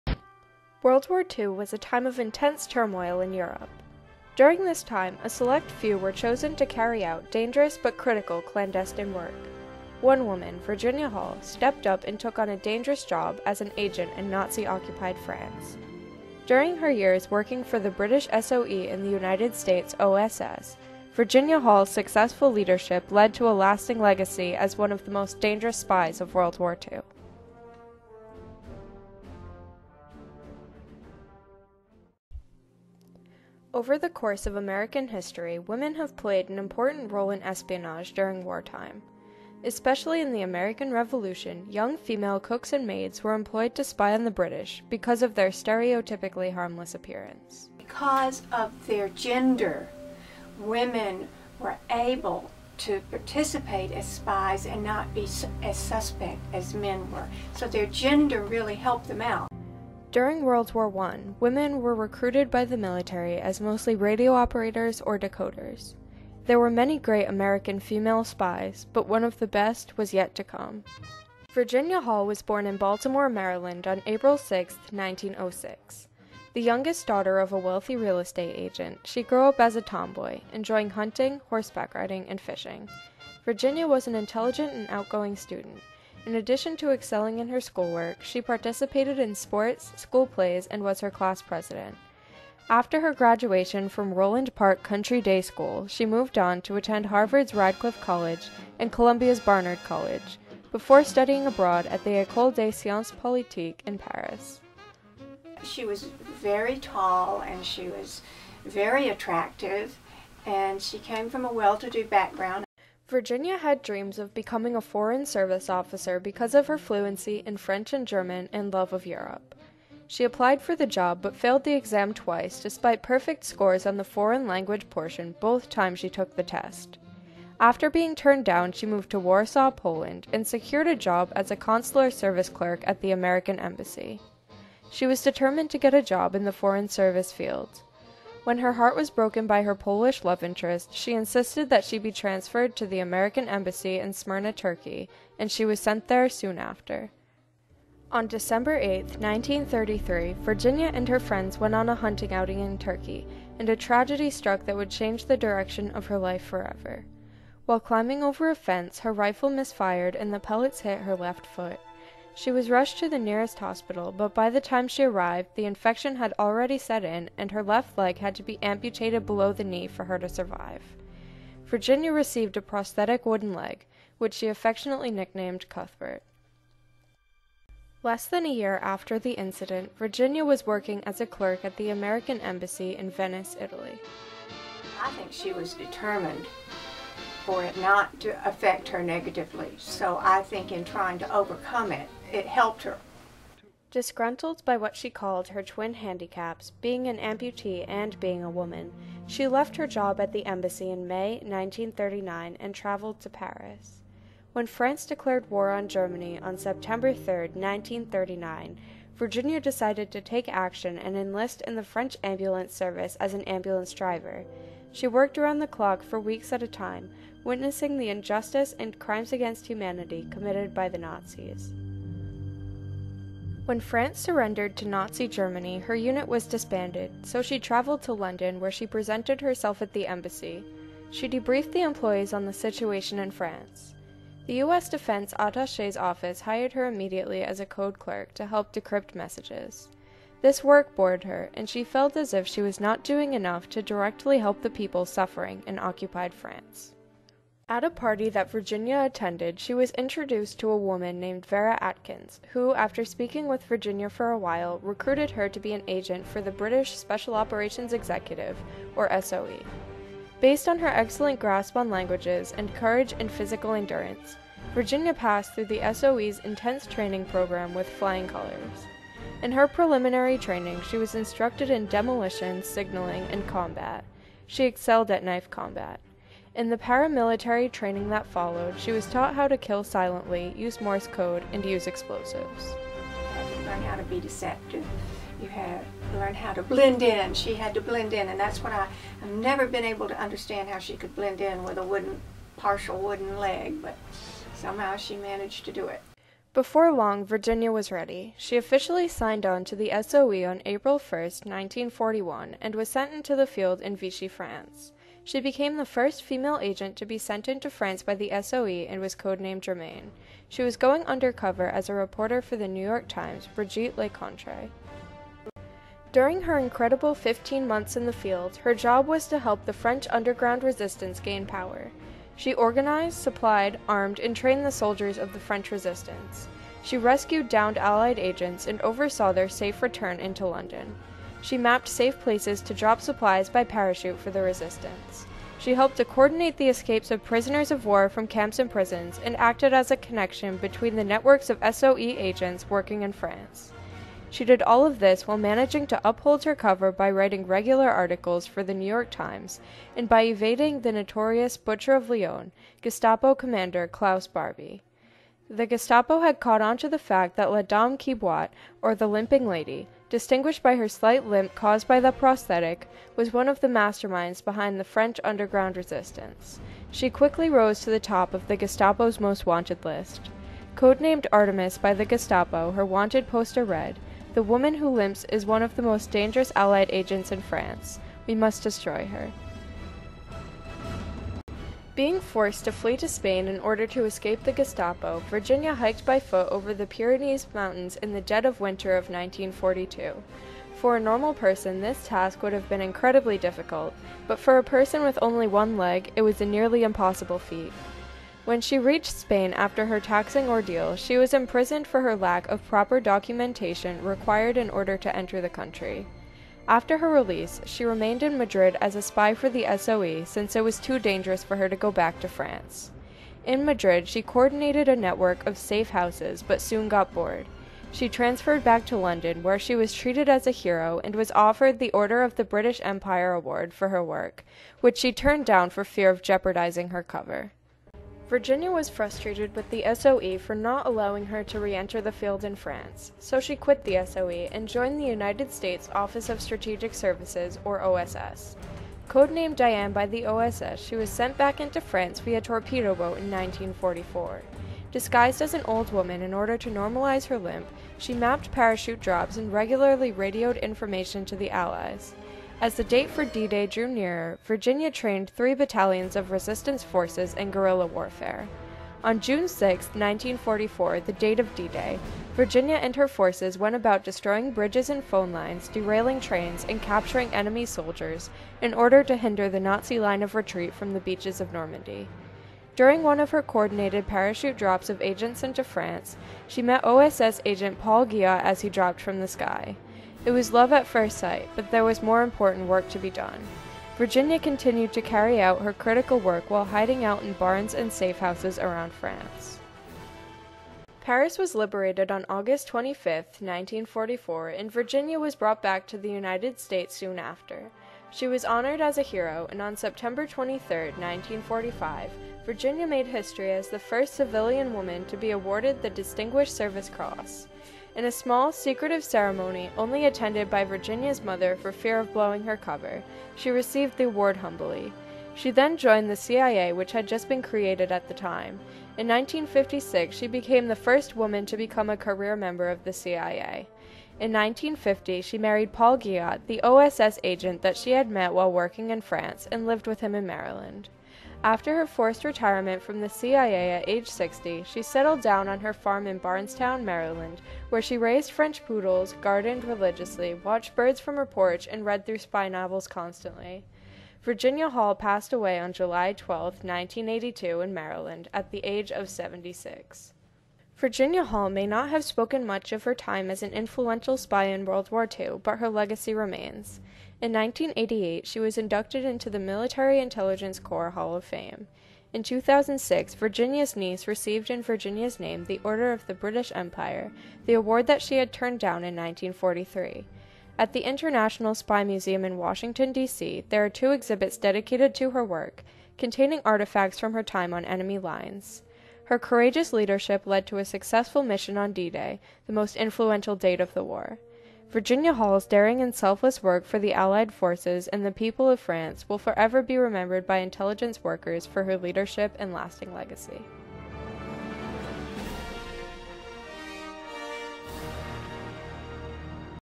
The Limping Lady: The Incredible Leadership of Virginia Hall (NHD 2015)
My documentary for National History Day.